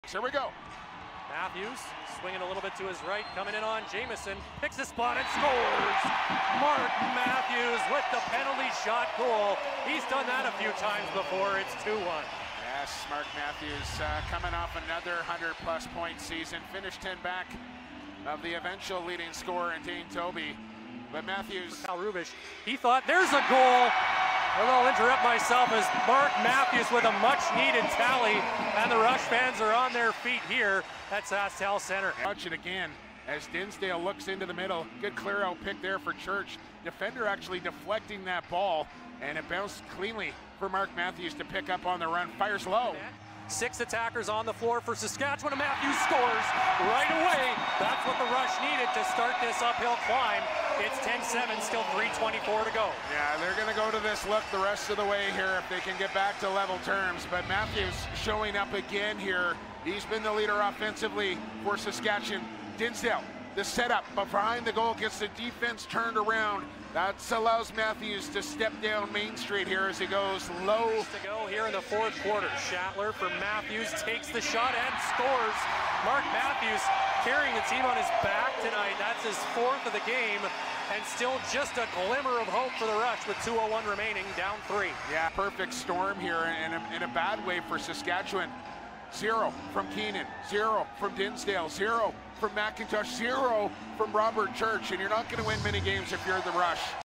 Matthews nets 4 goals in loss to New England
Mark Matthews opened the scoring for the Saskatchewan Rush with a penalty shot and kept up the energy all night with four goals in total against the New England Black Wolves.